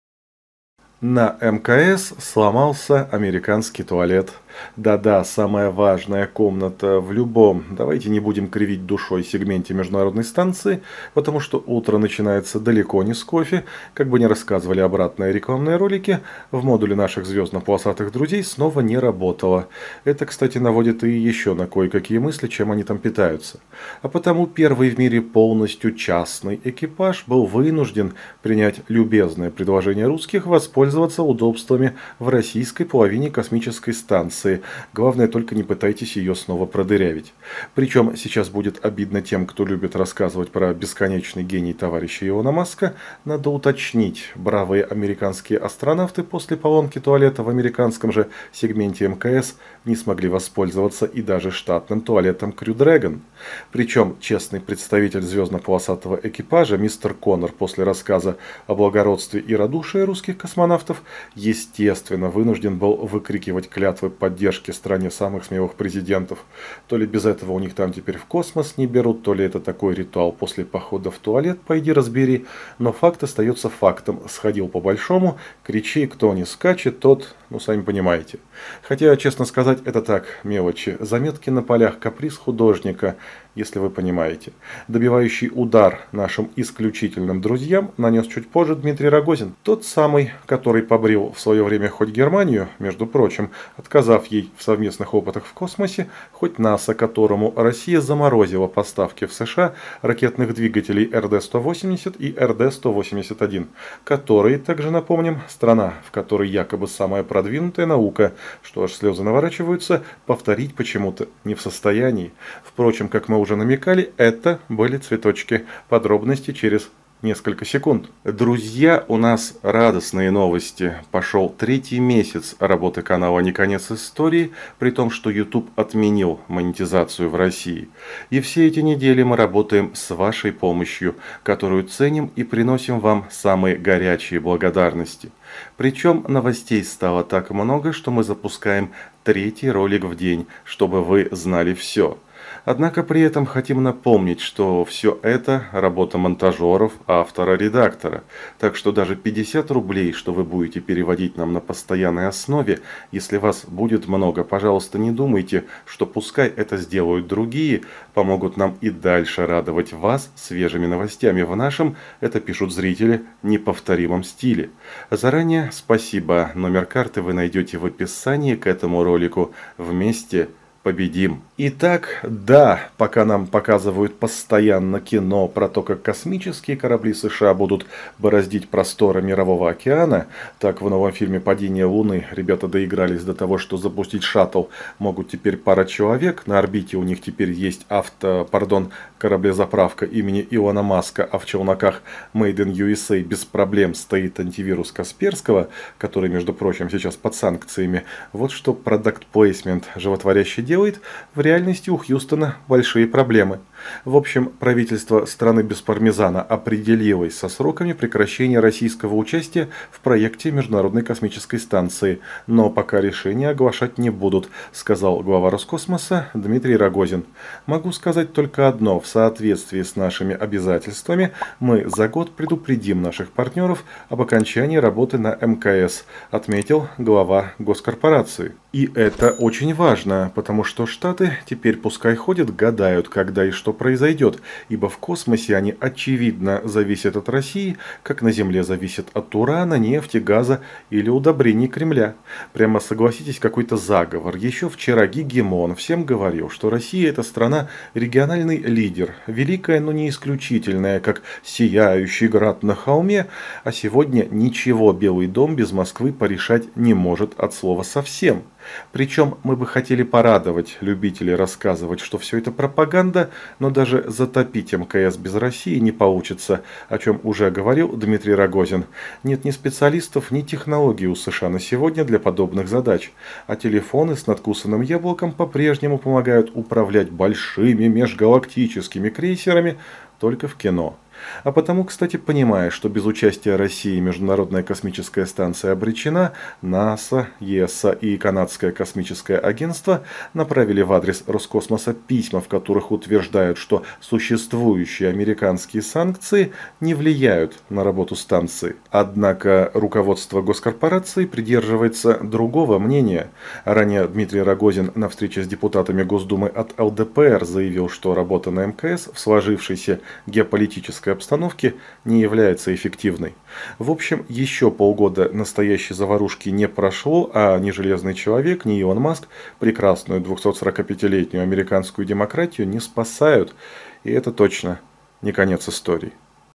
Срочно, Россия закрывает МКС! NASA затрясло, это провал!
На МКС сломался американский туалет… Да, да, самая важная комната в любом, давайте не будем кривить, сегменте международной станции, потому что утро начинается не с кофе, как бы не рассказывали обратное рекламные ролики – в модуле наших звездно-полосатых друзей снова не работала (это, кстати, наводит и еще кое на какие мысли, чем они там питаются).
А потому "первый в мире полностью частный экипаж" был вынужден принять  любезное предложение русских воспользоваться удобствами в российской половине космической станции.
Причем, сейчас будет обидно тем, кто любит рассказывать про бесконечный гений товарища Илона Маска, надо уточнить: бравые американские астронавты после поломки туалета в американском же сегменте МКС не смогли воспользоваться и даже штатным туалетом Crew Dragon.